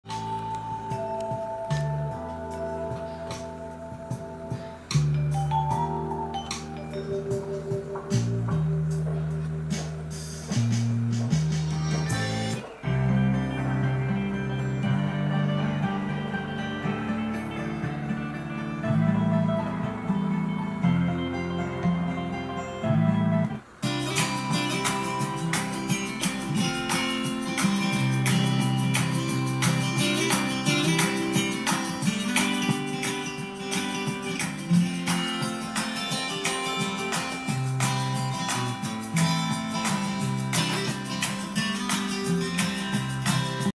Palladium vintage deck as digital music player - MP3 Tapeless Deck Project
Classic vintage Palladium - work in progress. It's a part of my Tapeless Deck Project, where I convert damaged decks into digital music machines.

Each Tapeless Deck is full of features:
• MP3 and FLAC player with animated cassette simulation
• Internet radio
• Spotify, Tidal, Deezer, Pandora etc. player (if You have an account)
• FM radio
• wireless music upload and management
• touch control
• internal storage for music files